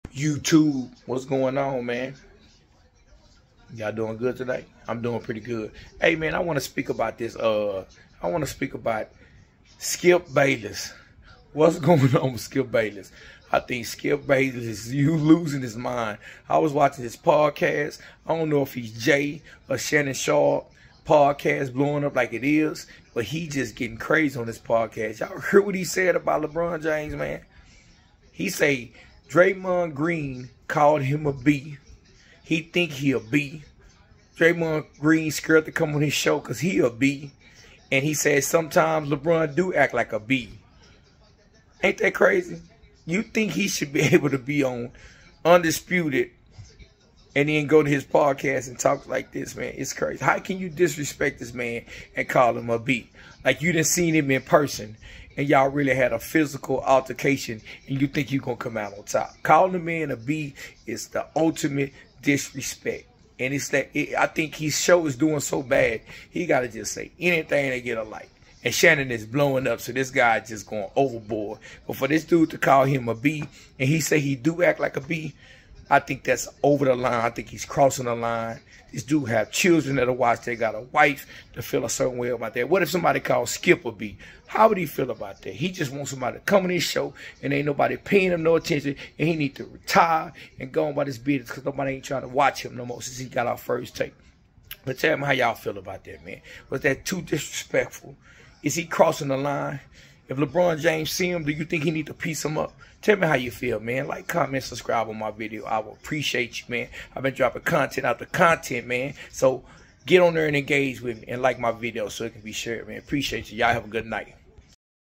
Is Skip Bayless Losing His Mind? The Disrespect Towards LeBron James
In this video, we discuss Skip Bayless' controversial statements about LeBron James on his podcast. Join the conversation and share your thoughts on whether Skip went too far.